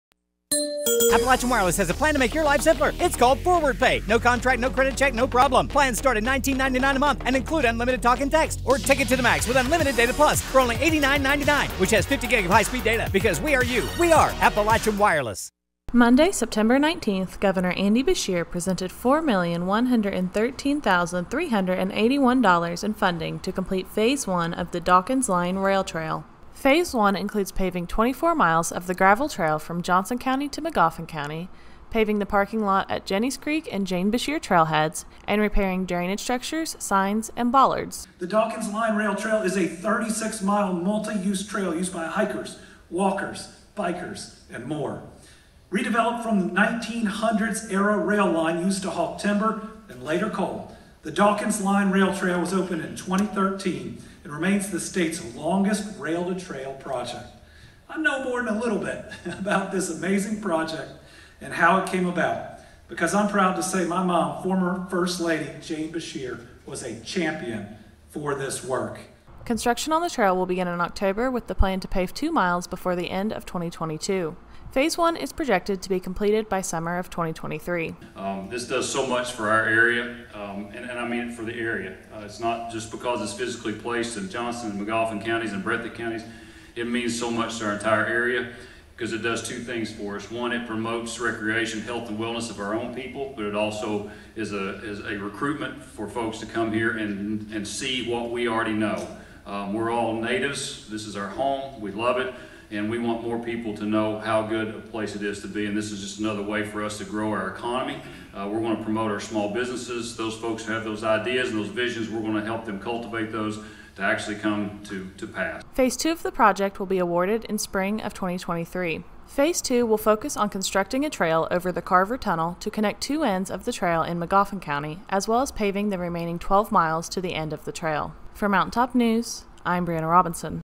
Governor Beshear Presents $4.1 Million Check for Dawkins Line Rail Trail
On Monday, September 19, Kentucky Governor Andy Beshear presented a check for funding the Dawkins Line Rail Trail.